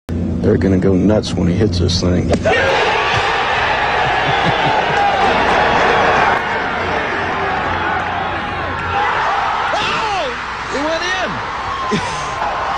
Tiger Woods Boccia Shot 🔵⚪️🔴
“They’re going to go nuts when he hits this thing”.
Daniel Michel makes an absolute precision boccia shot that rolls up closest to the Jack. It stops next to the Jack at perfect pace. A hole-in-one boccia style.

Accessible description: Australia’s Daniel Michel makes a shot by using an instrument he holds in his mouth. He pushes the ball with the tool which then rolls down the ramp to stop beside the white ball, the Jack.